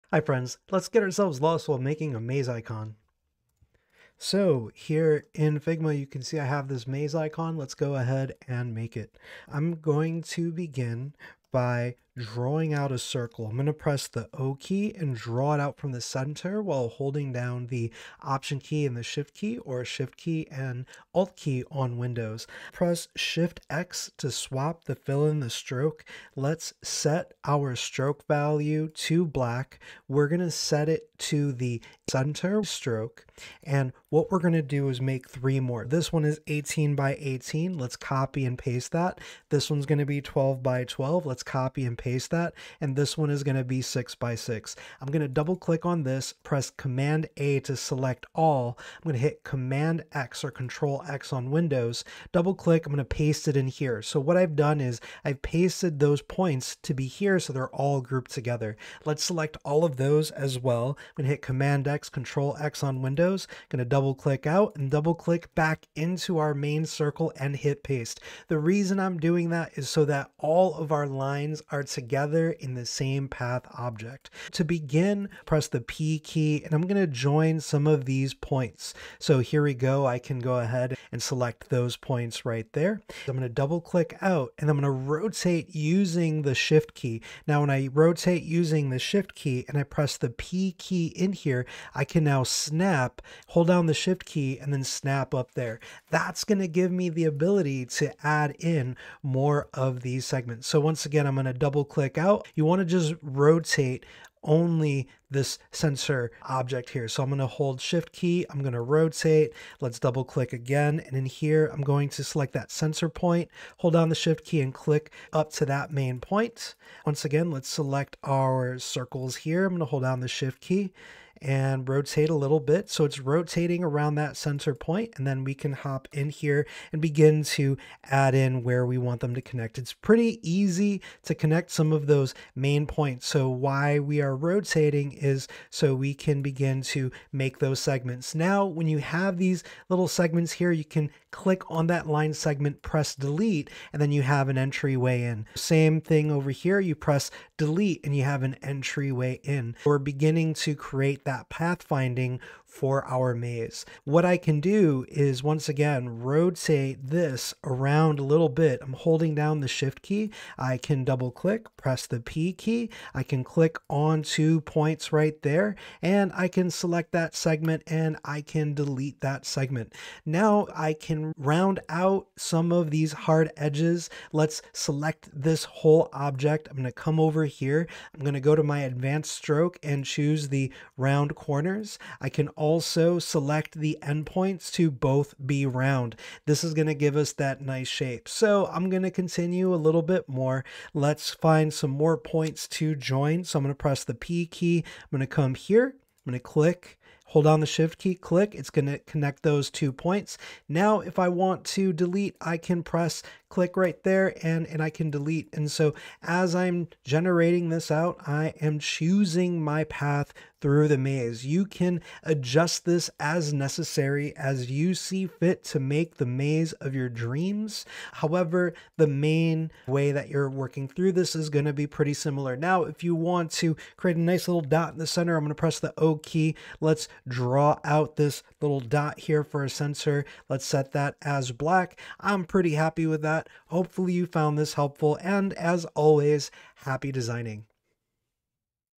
Episode 6: Making maze icons in Figma
Making maze icons in Figma. Today we rely on vector networks and rotation to create a circular maze icon.